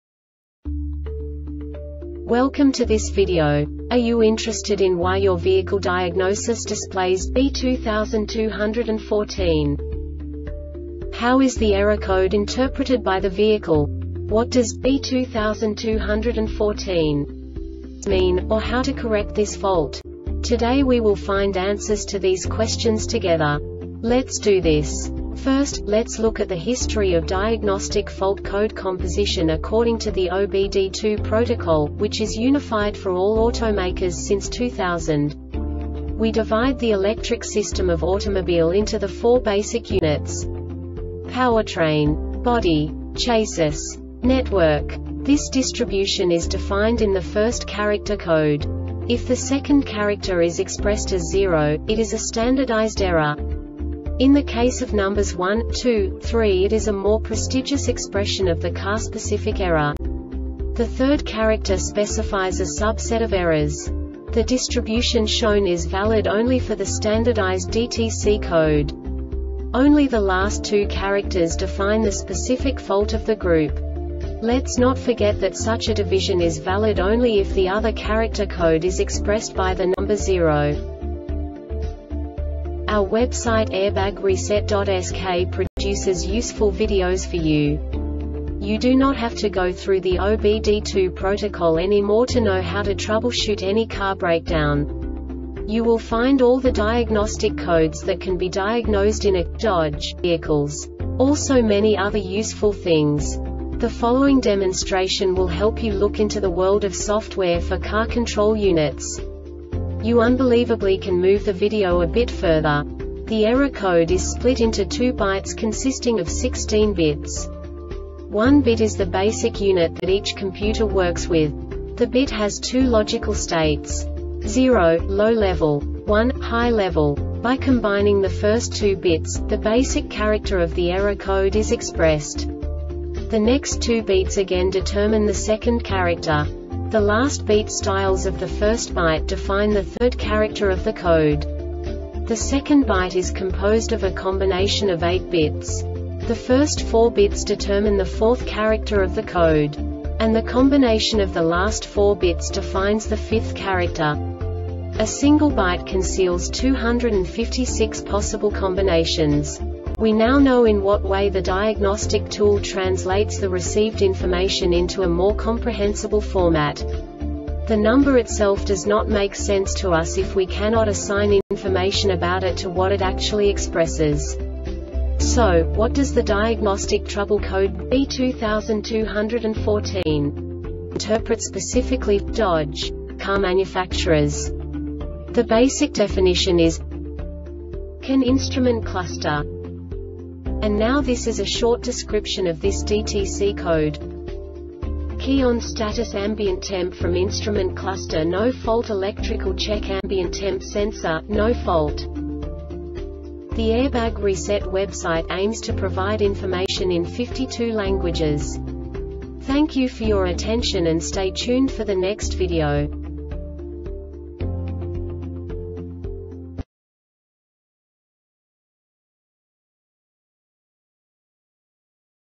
DTC Dodge B2214 Short Explanation
Contents:
0:21 Basic DTC analysis according to OBD2 protocol standard.
1:48 Insight into programming
2:58 A diagnostically understandable description of the Diagnostic Trouble Code
3:05 Basic error definition

Make:
Dodge
Code:
B2214
Definition:
CAN: Instrument cluster
Description:
key onstatus ambient temp from instrument cluster no faultelectrical check ambient temp.sensor,no fault